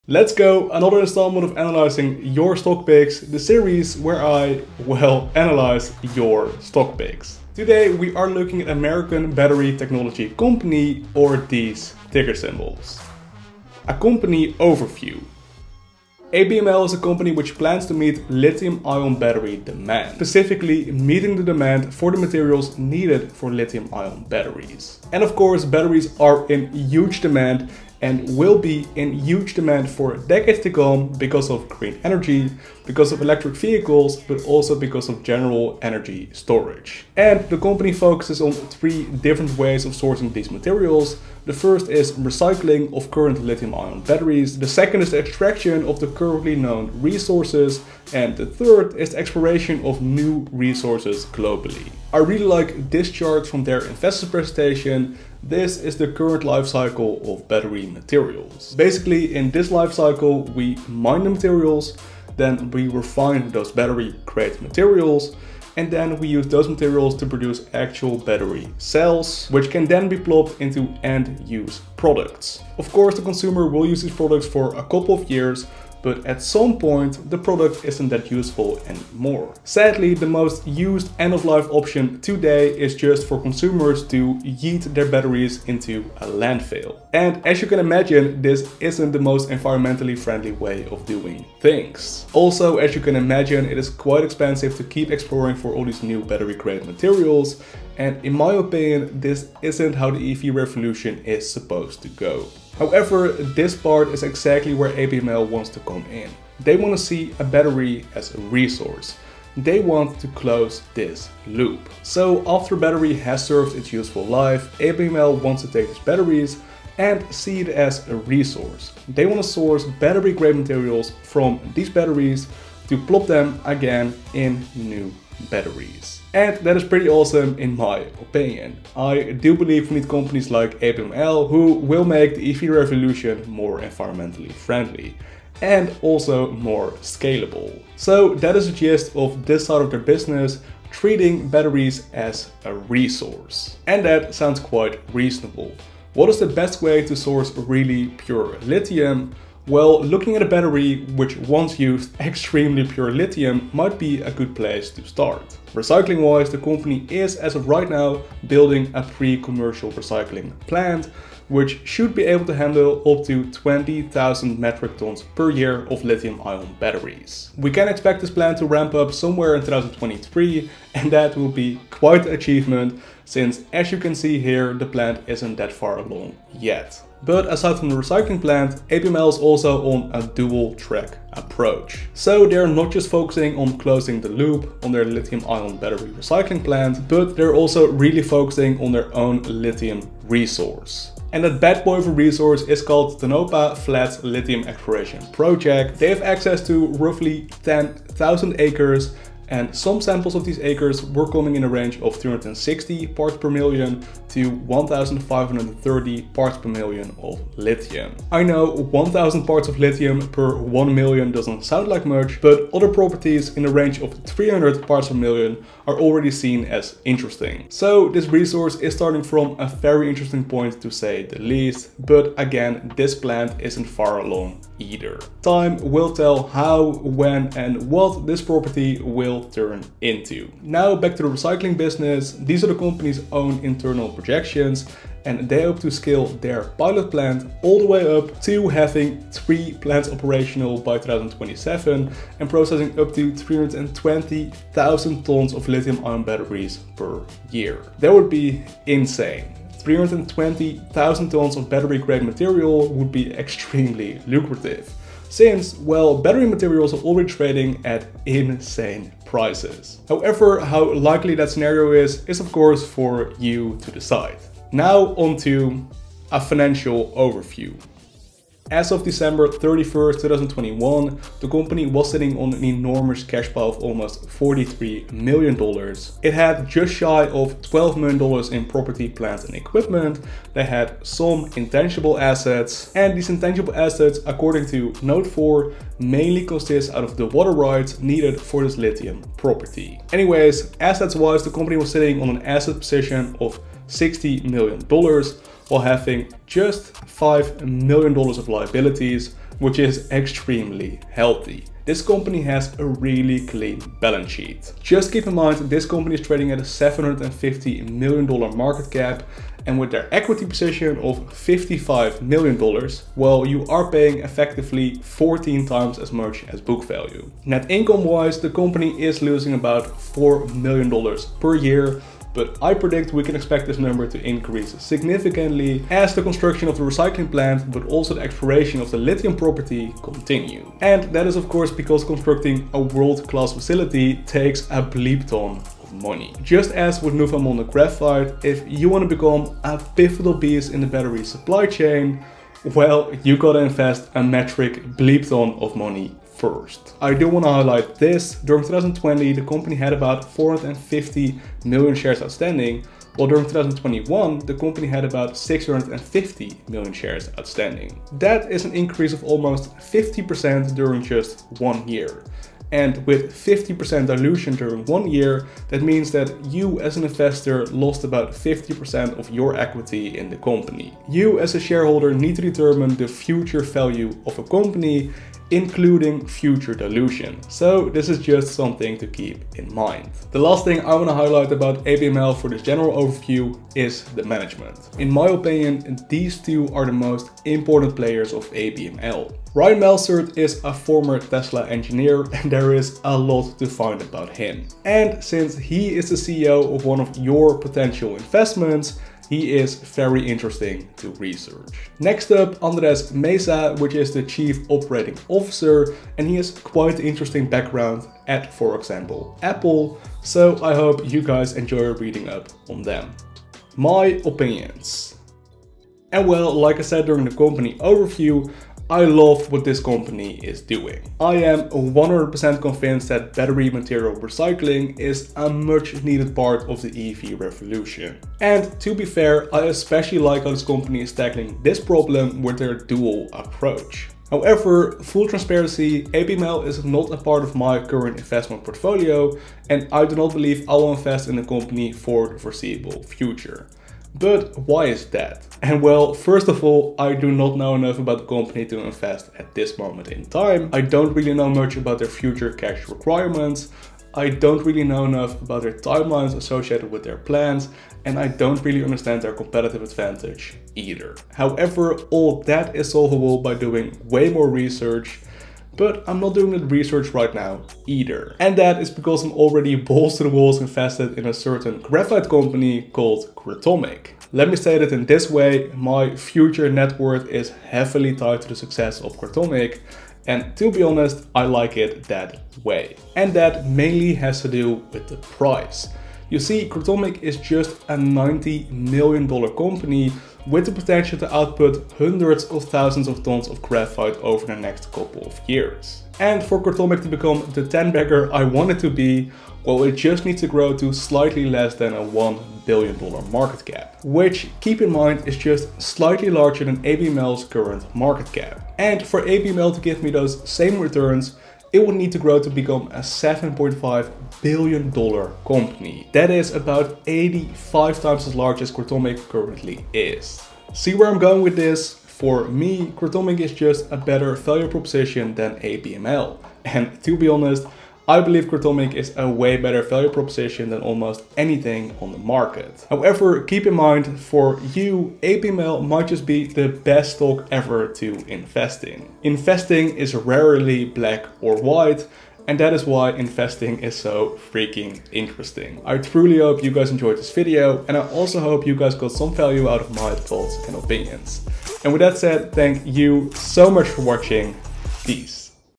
The Truth About ABML… | Recycling Lithium-Ion Batteries Stock
Let’s go baby! Another instalment of analysing your stock picks, the series where I, well, analyse your stock picks! Today we are looking at the American Battery Technology Company or ticker symbol ABML. American Battery Technology Company is a stock which revolves all around electric vehicles and recycling, specifically the recycling of battery grade materials.

00:00 – Intro
00:13 – A Company Overview
04:26 – A Financial Overview
06:53 – My Opinions on ABML